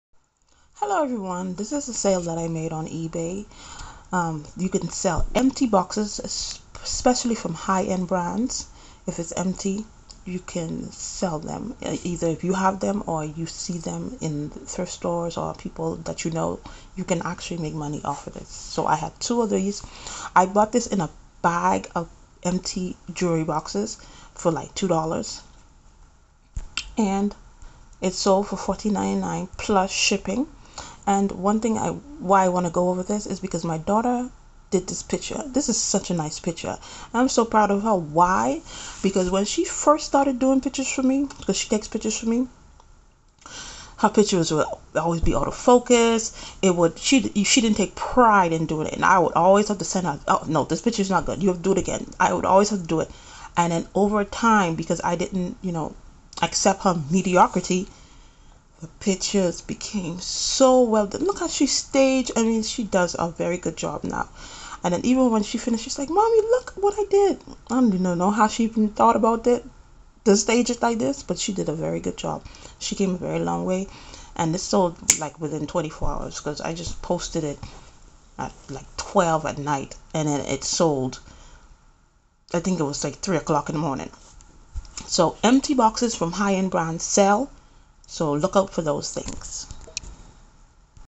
Selling Empty Boxes on eBay - My Daughter Taking Good Pictures Now
Another easy thing to sell online is empty boxes from high end brands. If you have any you could make quick money with them. My daughter has really improved on taking pictures she has came a long way.